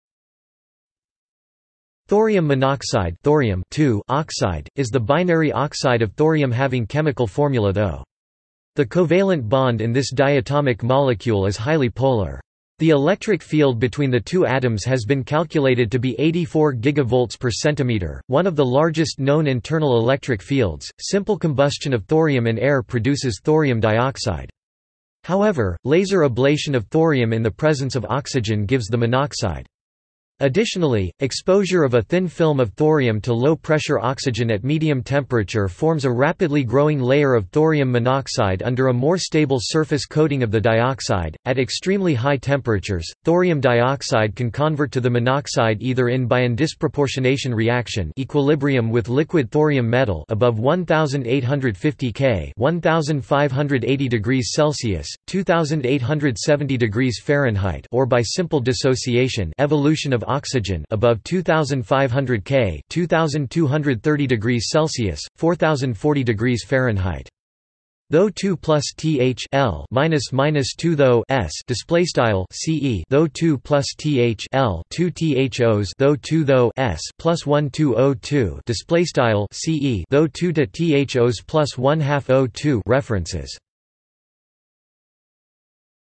Thorium monoxide | Wikipedia audio article
This is an audio version of the Wikipedia Article:
Thorium monoxide

SUMMARY
Thorium monoxide (thorium(II) oxide), is the binary oxide of thorium having chemical formula ThO. The covalent bond in this diatomic molecule is highly polar. The electric field between the two atoms has been calculated to be 84 gigavolts per centimeter, one of the largest known internal electric fields.Simple combustion of thorium in air produces thorium dioxide. However, laser ablation of thorium in the presence of oxygen gives the monoxide. Additionally, exposure of a thin film of thorium to low-pressure oxygen at medium temperature forms a rapidly growing layer of thorium monoxide under a more-stable surface coating of the dioxide.At extremely high temperatures, thorium dioxide can convert to the monoxide either in by an disproportionation reaction (equilibrium with liquid thorium metal) above 1,850 K (1,580 °C; 2,870 °F) or by simple dissociation (evolution of oxygen) above 2,500 K (2,230 °C; 4,040 °F).

            ThO

              2

          Th

          2

          ThO

    {\displaystyle {\ce {ThO2 + Th(l) = 2 ThO(s)}}}

            ThO

              2

          ThO

              1
              2

            O

              2

    {\displaystyle {\ce {ThO2 - ThO(s) + 1/2 O2}}}